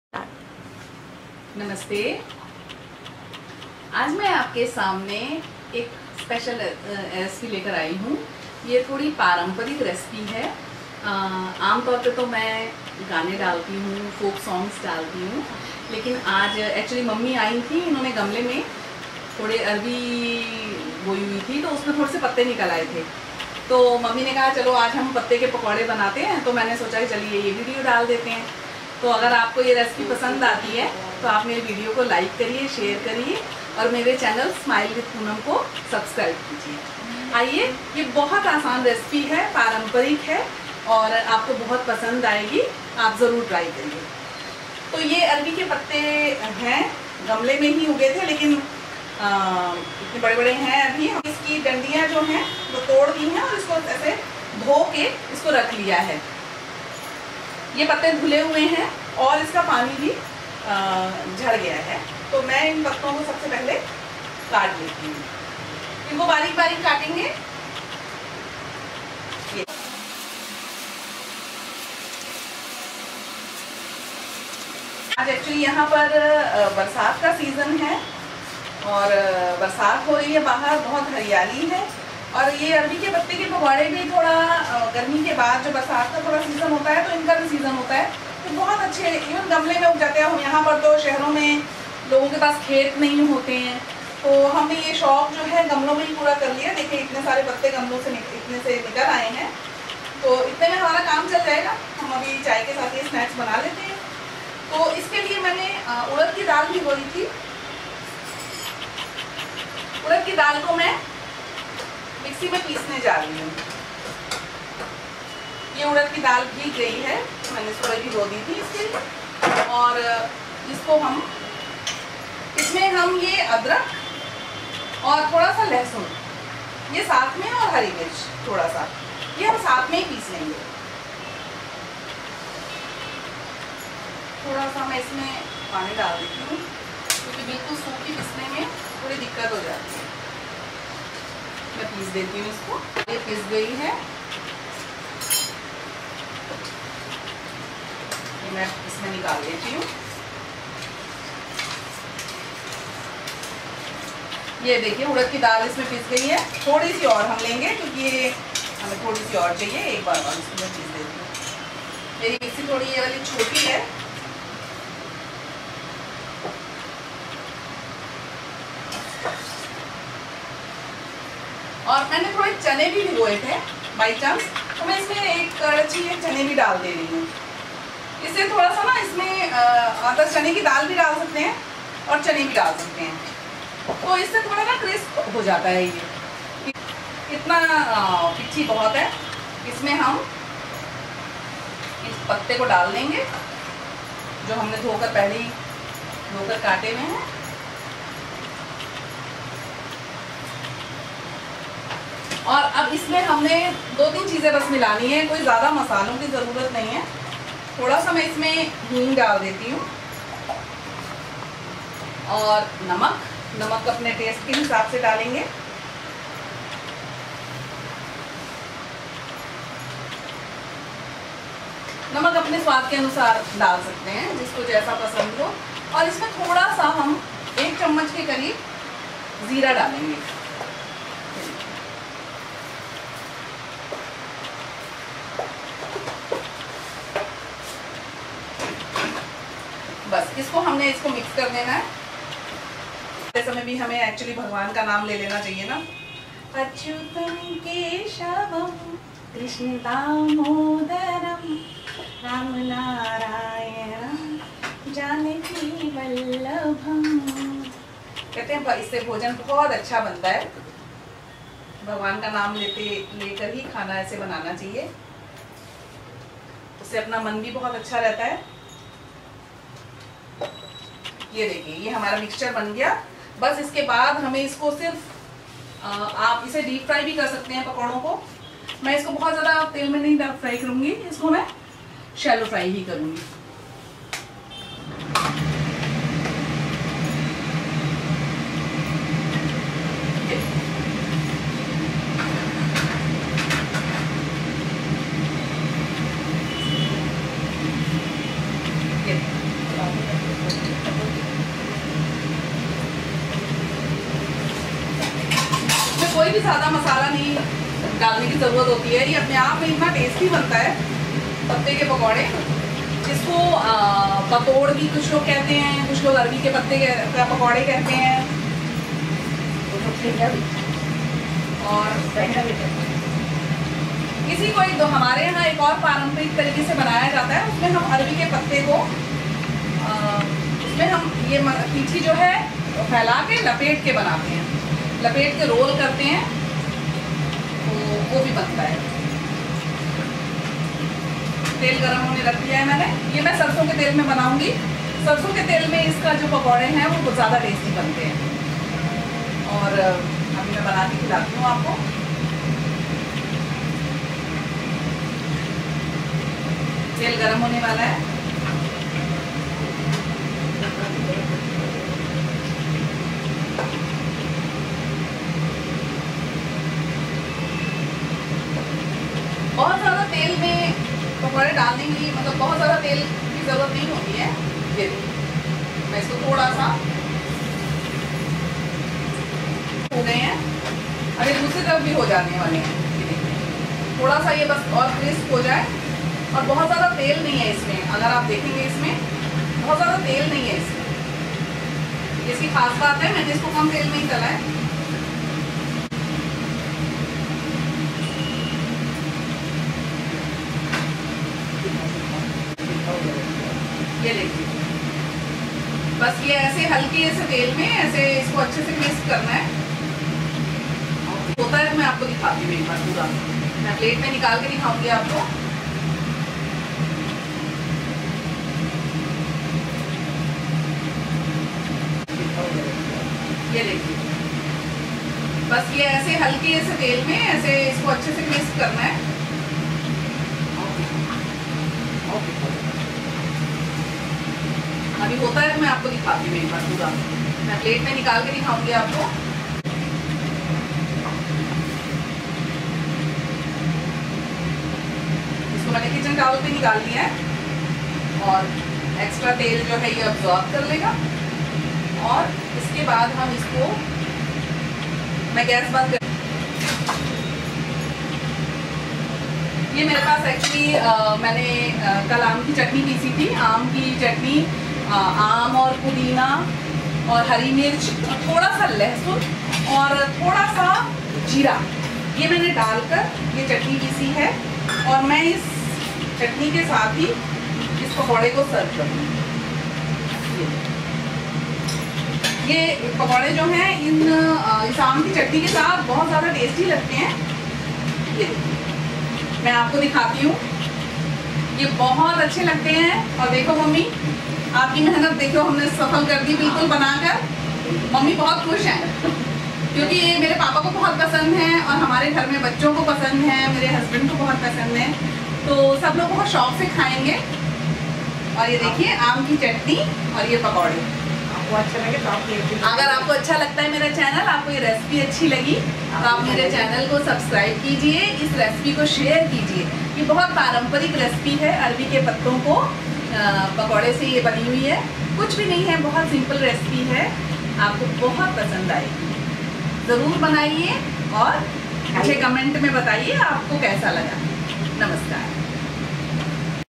Arbi Patte Ke Pakore [Patore] [colocasia leaves fritters] -Poonam Srivastava-smilewithpoonam
Here I am presenting a very tasty and Traditional recipe- Arbi  ke Patte Ke Pakore , also known as "Patore" [colocasia leaves fritters].It really comes out very crunchy, and delicious.It is very popular Indian Recipe.I hope you all like this video.
INGREDIENTS---
Arbi Leaves
Urad Dal [black lentil] paste
garlic cloves-7-8
hing   [asafetida]
salt

For mango chutney-
raw mango
salt
mint leaves
jeera [cumin seeds]
garlic cloves
green chillies as per taste

GOD BLESS ALL OF US.
smile with poonam
smilewithpoonam